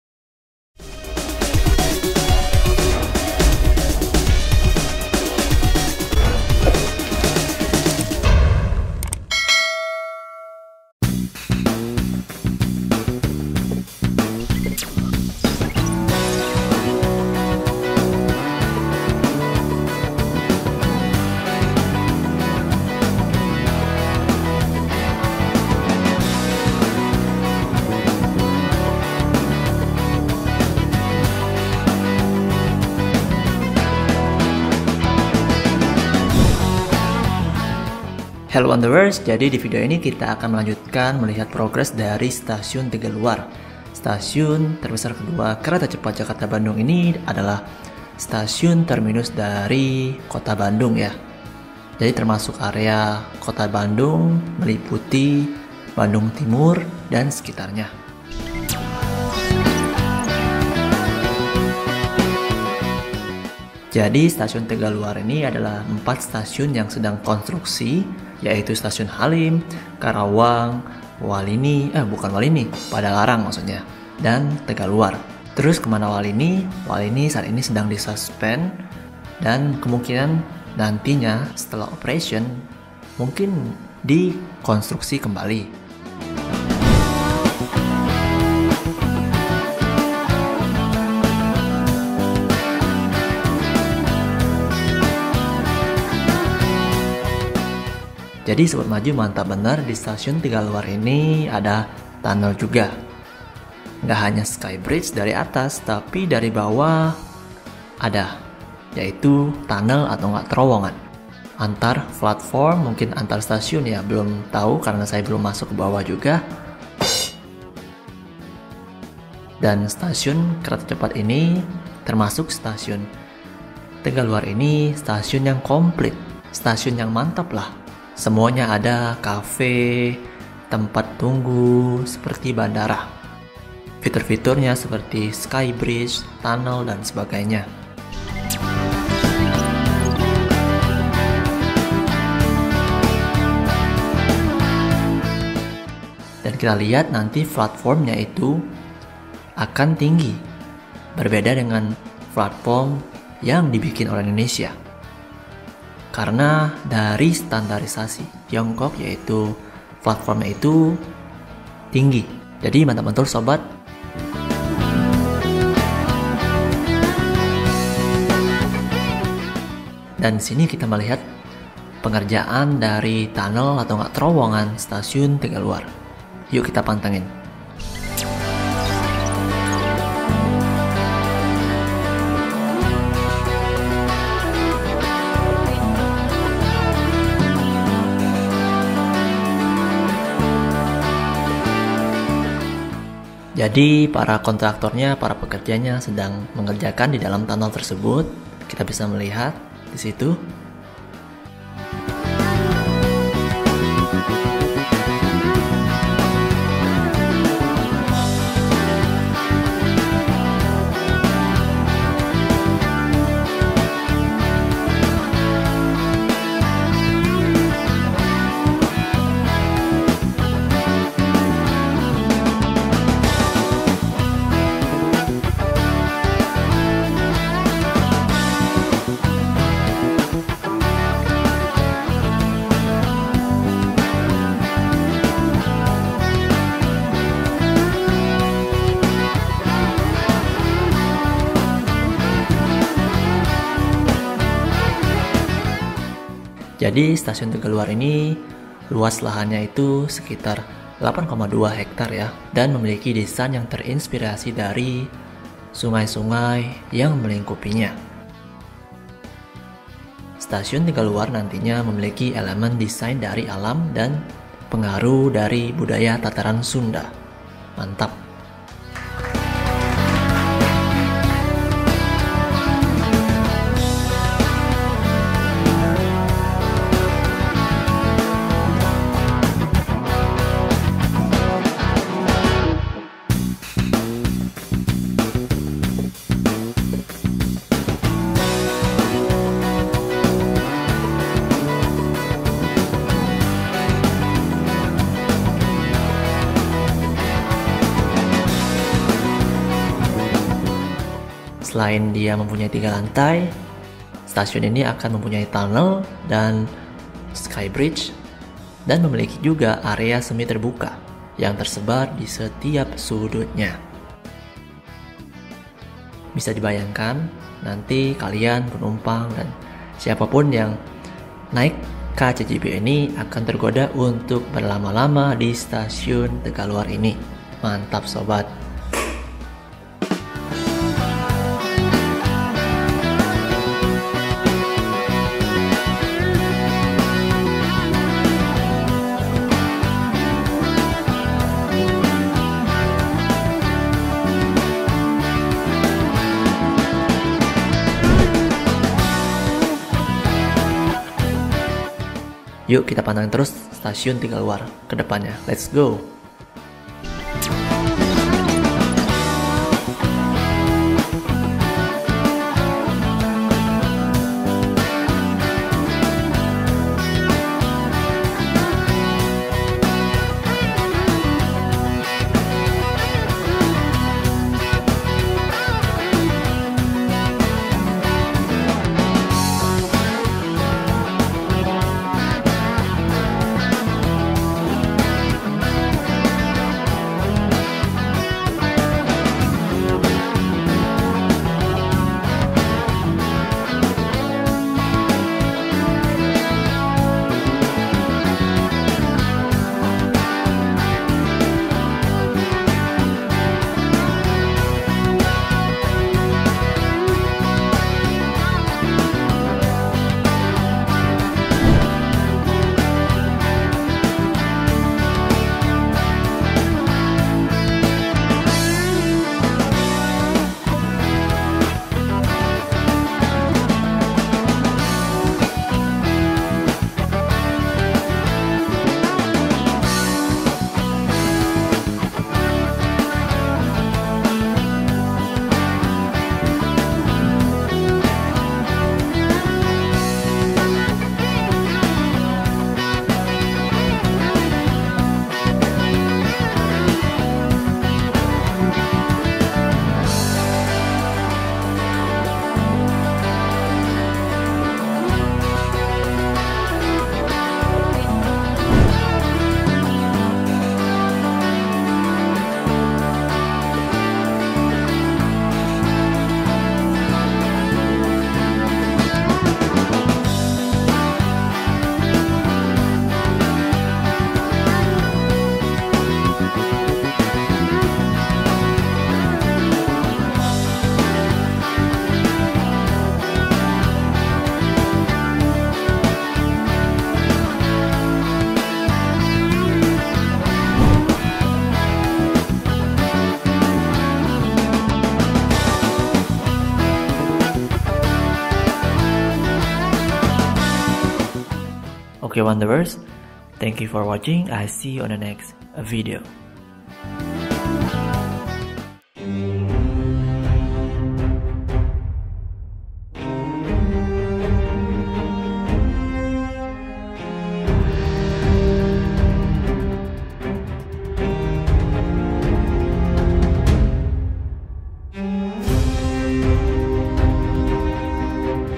Wow Ada Tunnel Di Stasiun, Pengerjaan Terowongan Stasiun Kereta Cepat Jakarta Bandung Makin Lengkap
Indonesia Revolution

Hello wanderer di video ini saya akan melihatkan kalian tunnel stasiun tegalluar kereta cepat jakarta bandung

🔥  SUPPORT CHANNEL THE WANDERER INDONESIA JELAJAH INDONESIA🔥

0:00 -- Intro
0:13 -- Main